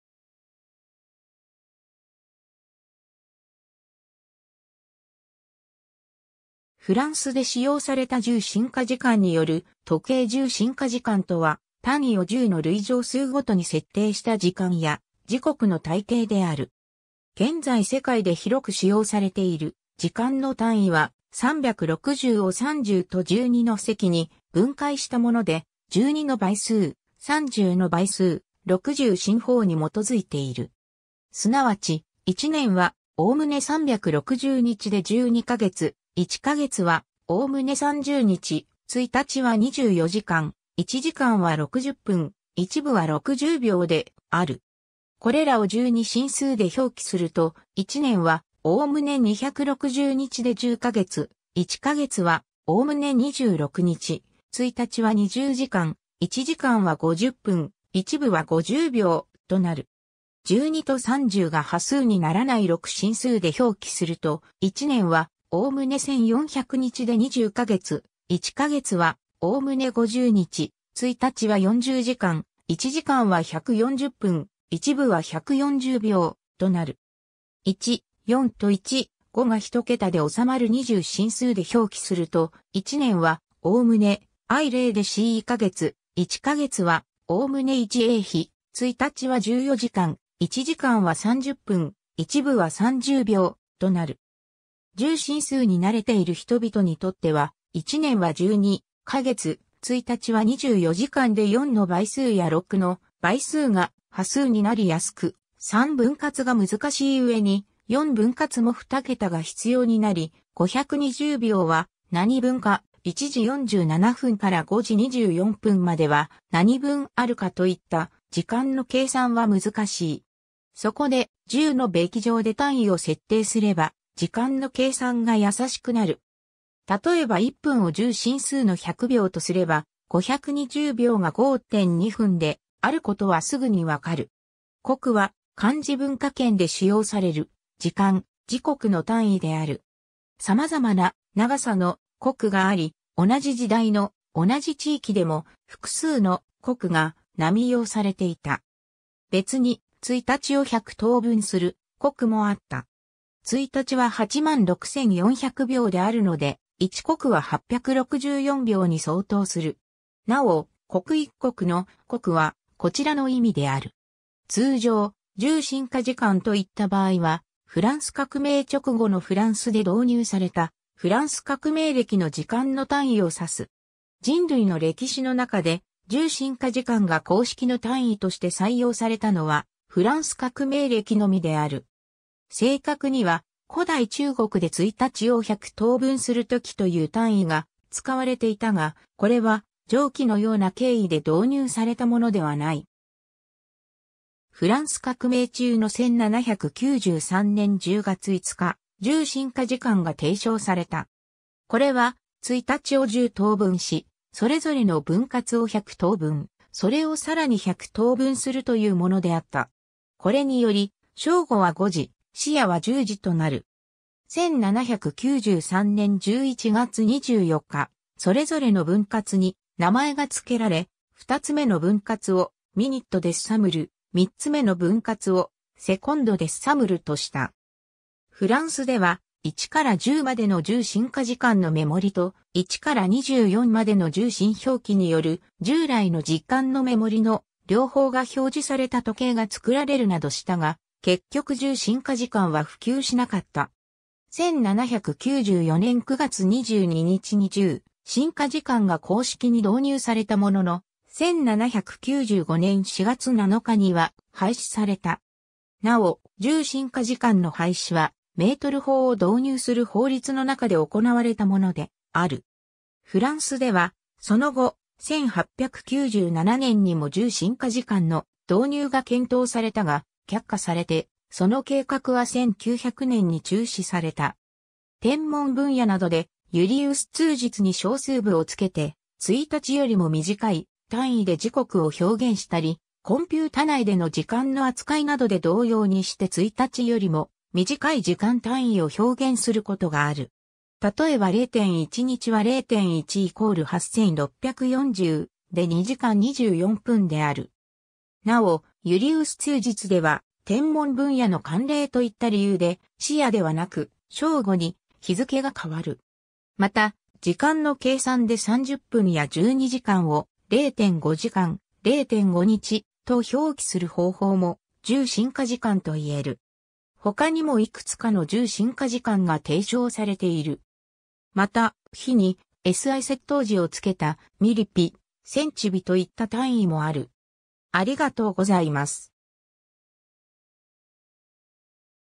十進化時間
フランスで使用された十進化時間による時計 十進化時間（じっしんかじかん）とは、単位を十の累乗数ごとに設定した時間や時刻の体系である。
 現在世界で広く使用されている時間の単位は、三百六十を三十と十二の積に分解した物で、十二の倍数、三十の倍数、六十進法に基づいている。
即ち、1年は概ね360日で12か月、1か月は概ね30日、1日は24時間、1時間は60分、1分は60秒である。
これらを十二進数で表記すると「1年は概ね260日で10か月、1か月は概ね26日、1日は20時間、1時間は50分、1分は50秒」となる。
十二と三十が端数にならない六進数で表記すると、「1年は概ね1400日で20か月、1か月は概ね50日、1日は40時間、1時間は140分、1分は140秒」となる。
1/4と1/5が一桁で収まる二十進数で表記すると、「1年は概ねI0日でCか月、1か月は概ね1A日、1日は14時間、1時間は30分、1分は30秒」となる。
 十進数に慣れている人々にとっては、「1年は12か月」「1日は24時間」で四の倍数や六の倍数が端数になり易く、三分割が難しい上に、四分割も二桁が必要になり、「520秒は何分か」「1時47分から5時24分までは何分あるか」といった時間の計算は難しい。
そこで、十の冪乗で単位を設定すれば、時間の計算が易しくなる。
例えば1分を十進数の100秒とすれば、520秒が5.2分（5分20秒）であることはすぐに判る。
 刻は、漢字文化圏で使用される時間・時刻の単位である。
様々な長さの「刻」があり、同じ時代の同じ地域でも、複数の「刻」が並用されていた。
別に、1日を百等分する「刻」もあった。
1日は86400秒であるので、1刻は864秒（14分24秒）に相当する。
なお、「刻一刻」の「刻」はこちらの意味である。
 通常、「十進化時間」と言った場合は、フランス革命直後のフランスで導入された、フランス革命暦の時間の単位を指す。
人類の歴史の中で、十進化時間が公式の単位として採用されたのは、フランス革命暦のみである。
正確には、古代中国で1日を百等分する刻という単位が使われていたが、これは上記のような経緯で導入されたものではない。
 フランス革命中の1793年10月5日、十進化時間が提唱された。
これは、1日を十等分し、それぞれの分割を百等分、それをさらに百等分するというものであった。
これにより、正午は5時、子夜は10時となる。
1793年11月24日（共和暦2年霜月4日）、それぞれの分割に名前がつけられ、2つ目の分割（1/1000日）をminute décimale（英: decimal minute、十進化分）、3つ目の分割（1/100000日）をseconde décimale（英: decimal second、十進化秒）とした。
 フランスでは、1から10までの十進化時間の目盛りと、1から24までの十進表記による従来の時間の目盛りの、両方が表示された時計が作られるなどしたが、結局十進化時間は普及しなかった。
1794年9月22日（共和暦3年）に十進化時間が公式に導入されたものの、1795年4月7日（共和暦3年芽月18日）には廃止された。
なお、十進化時間の廃止は、メートル法を導入する法律の中で行われたものである。
 フランスではその後、1897年にも十進化時間の導入が検討されたが、却下されてその計画は1900年に中止された。
 天文分野などでユリウス通日に小数部を付けて、1日よりも短い単位で時刻を表現したり、コンピュータ内での時間の扱いなどで同様にして1日よりも短い時間単位を表現することがある。
たとえば0.1日は (24×60×60)×0.1 ＝ 8640 で2時間24分である。
なお、ユリウス通日では天文分野の慣例といった理由で、子夜ではなく正午に日付が変わる（通日が ?.0 になる）。
 また、時間の計算で三十分や十二時間を「0.5時間」「0.5日」と表記する方法も、十進化時間と言える。
 他にもいくつかの十進化時間が提唱されている。
 また、日にSI接頭辞をつけた「ミリ日」「センチ日」といった単位もある。